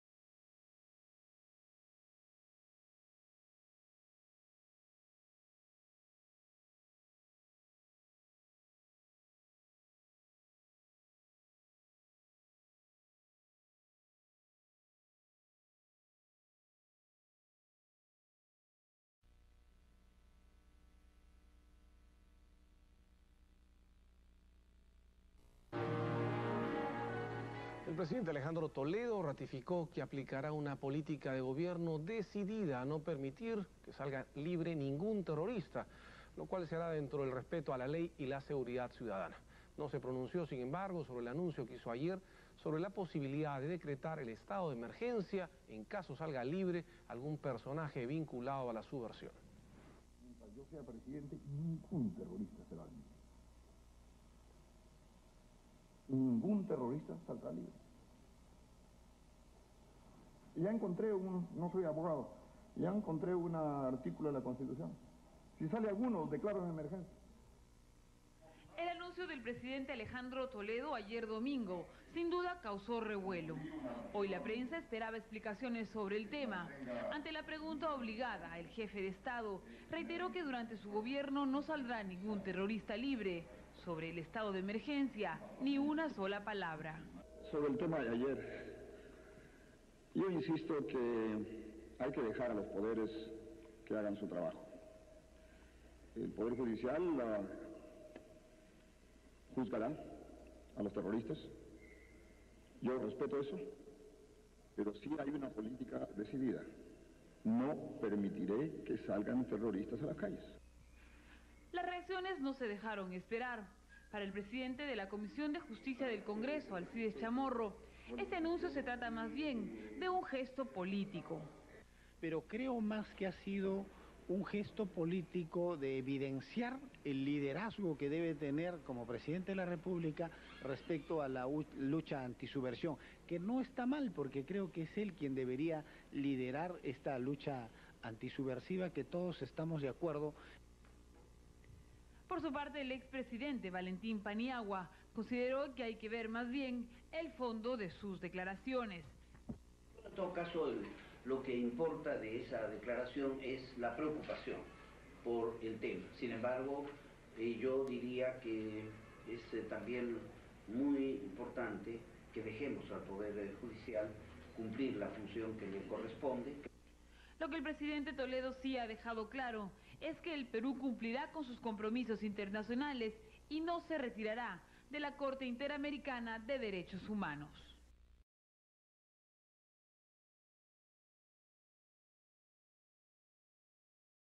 Alejandro Toledo insiste en reforma del poder judicial
Monitoreo televisivo
LUM - Centro de Documentación e Investigación